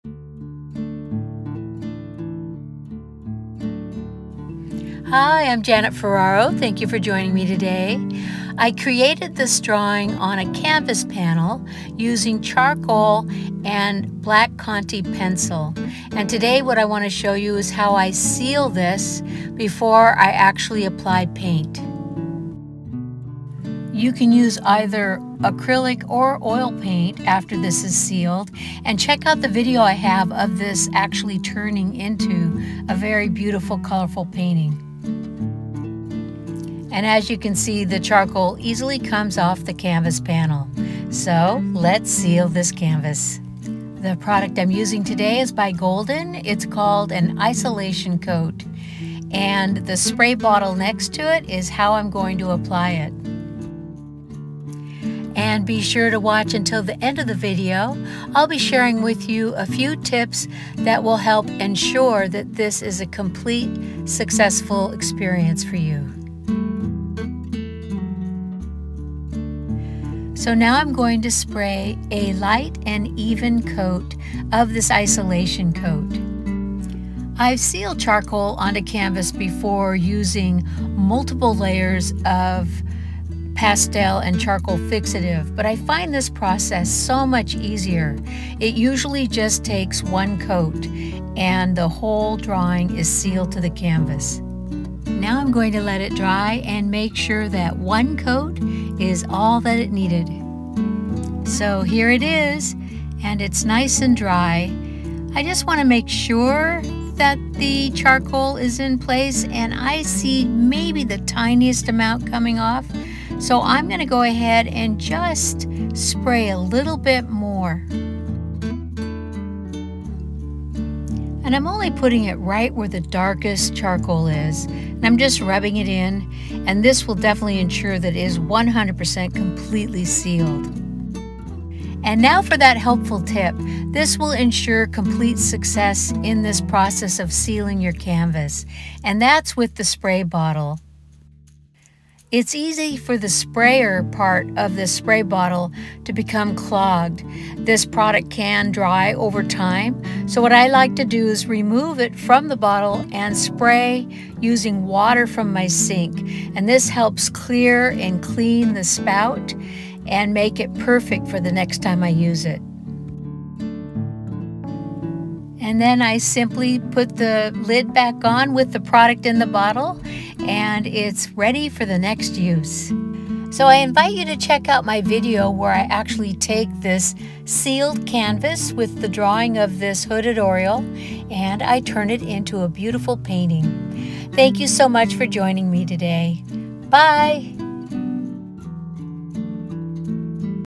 How To Seal Charcoal To A Canvas
Charcoal can be sealed successfully to your canvas. In this video I explain the step-by-step process of how I easily seal my charcoal drawing to my canvas. This works whether using a stretched canvas or a canvas panel. Once the charcoal is sealed, I can use acrylic paint or oil paint and create a beautiful painting over the sealed surface.
Janet Ferraro
Supply Link:
Isolation Coat